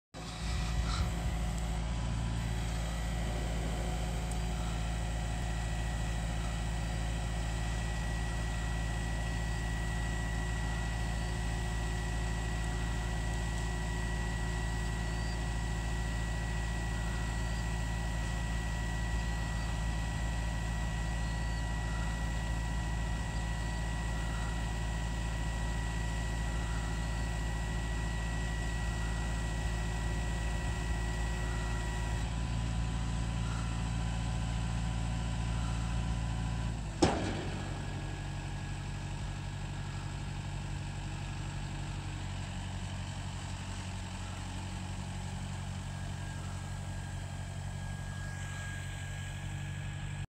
Kubota M7060 and a Hesston 5800 baler baling for the first time.
Kubota M7060
Hesston 5800 baler
This is brome grass and weeds. We put up about 20 bales a year to feed our registered Herefords.